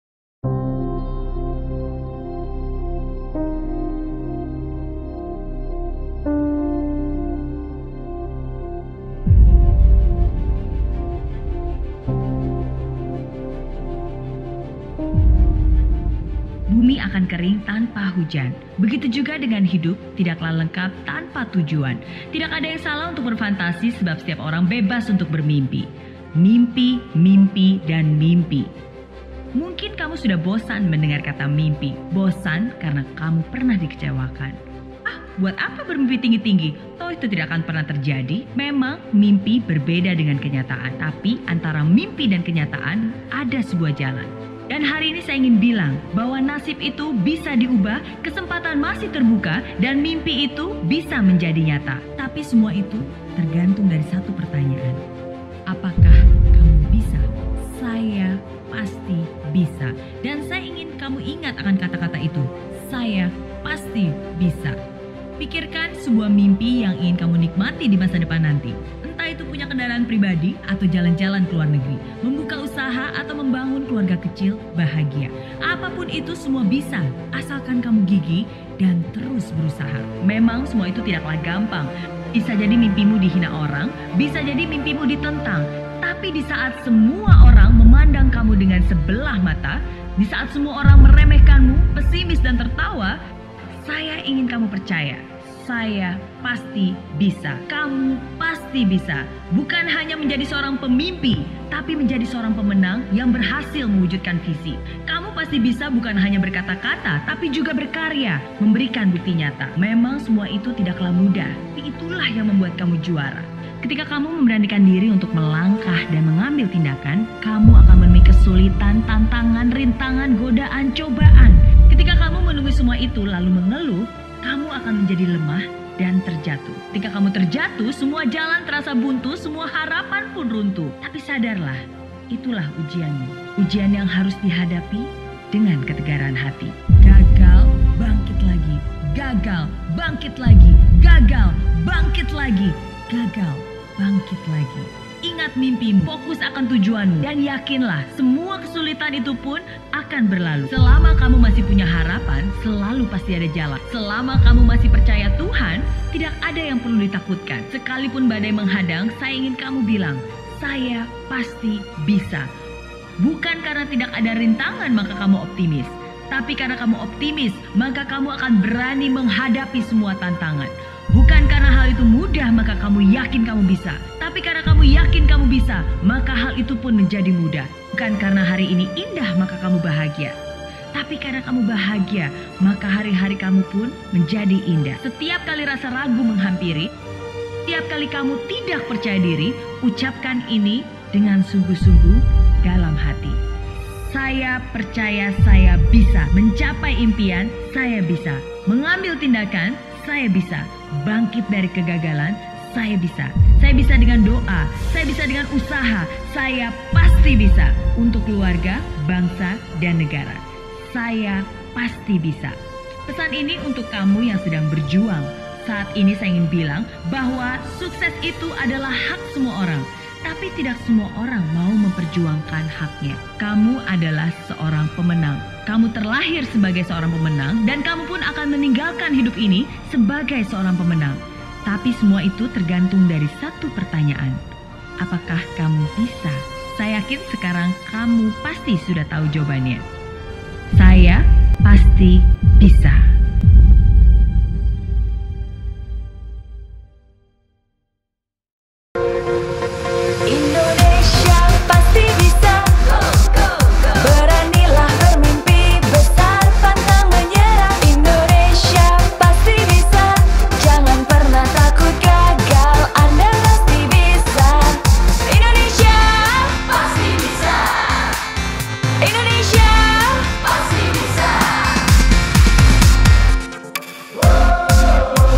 SAYA PASTI BISA (Video Motivasi) | Spoken Word | Merry Riana
Video Motivasi Merry Riana
Spoken Word | SAYA PASTI BISA

Siapa bilang nasib tidak bisa diubah?
Tetap yakin dan percaya bahwa, SAYA PASTI BISA!